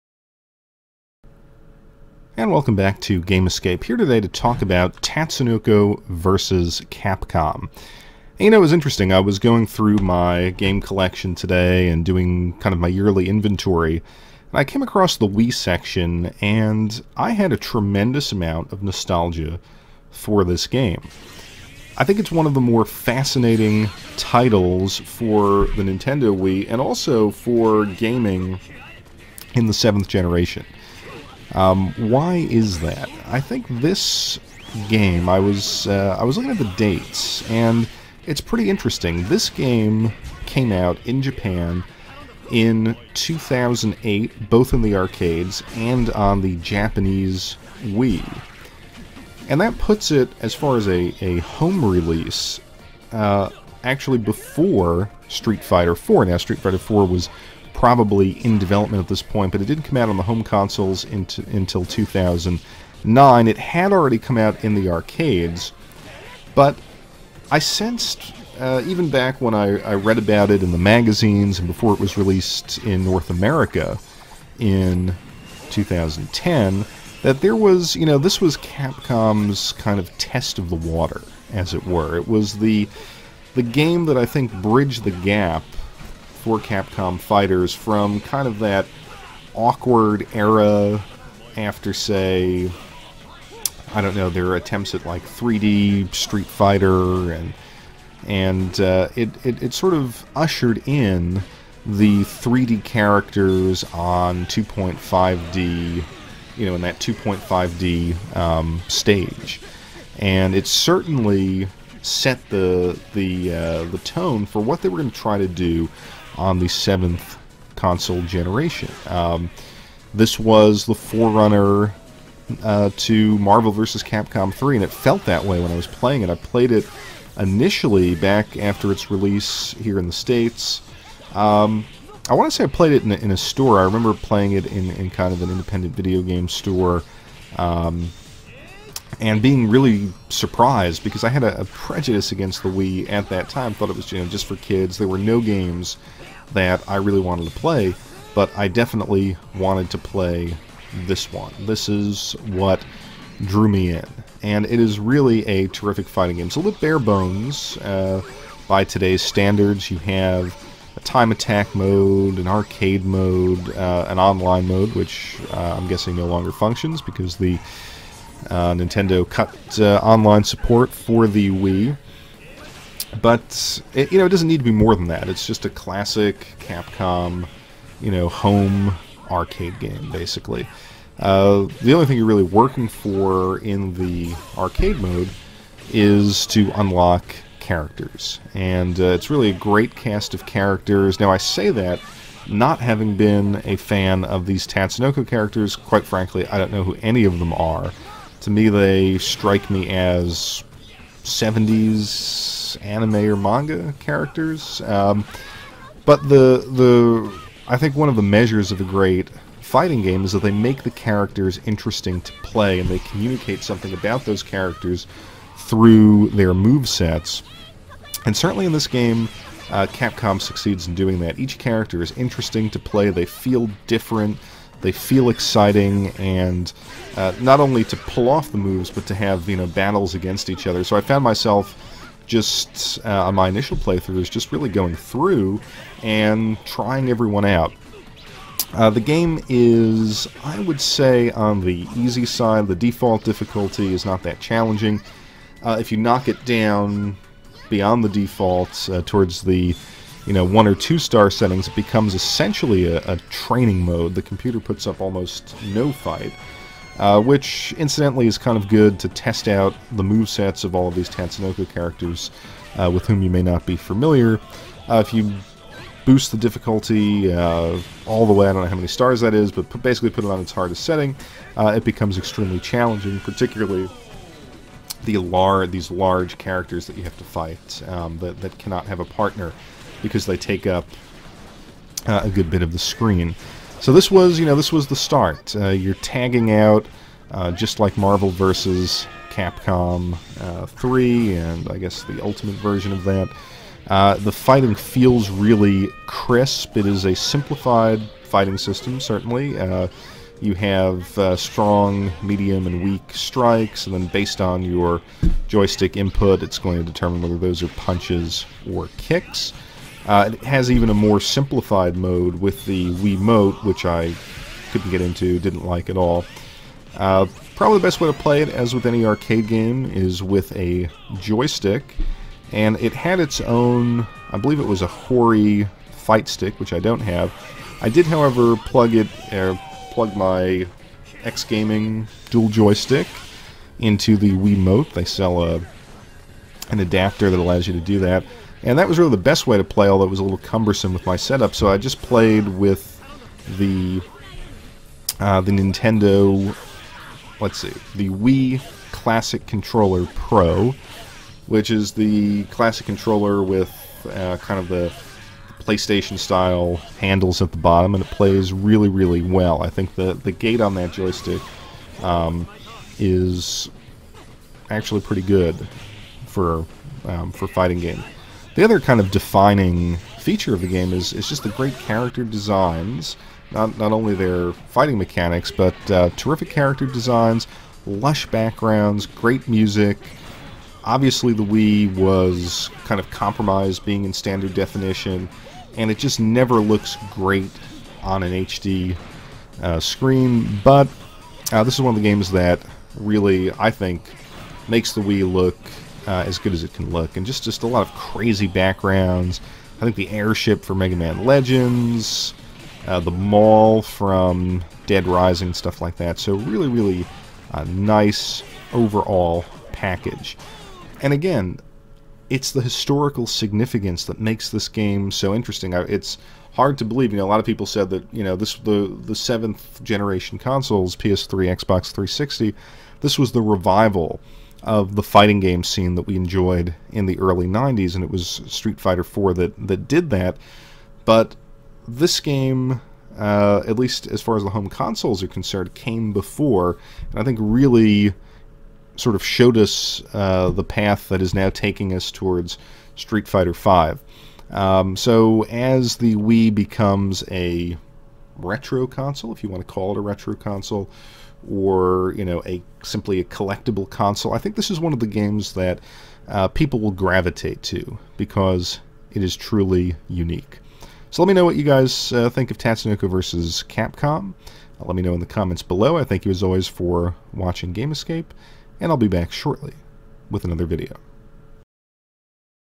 Tatsunoko vs. Capcom: Ultimate All Stars Review-Nintendo Wii
A look back on the significance of Tatsunoko vs. Capcom on the Wii.